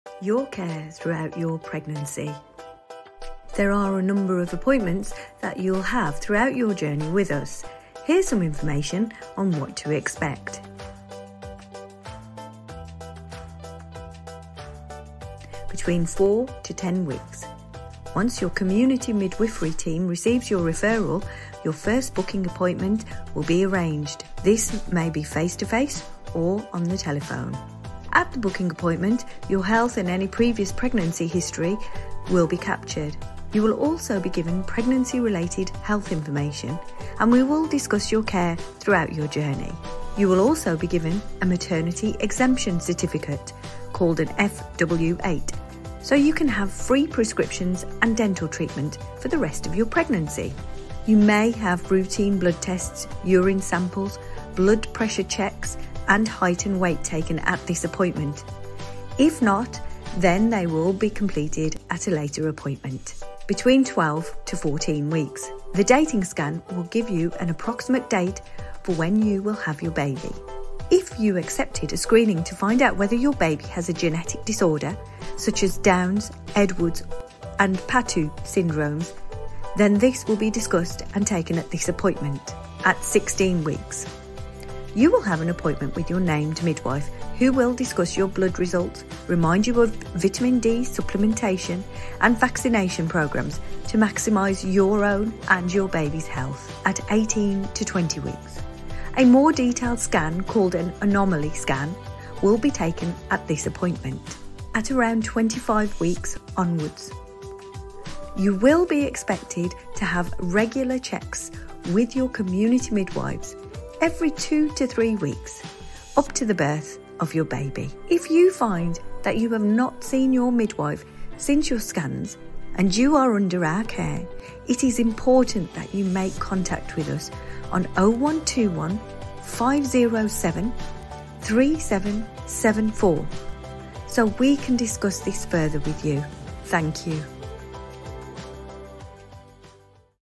Your Care Throughout Your Pregnancy | Maternity
In this video you will be find out what will happen at each of your appointments with your midwife.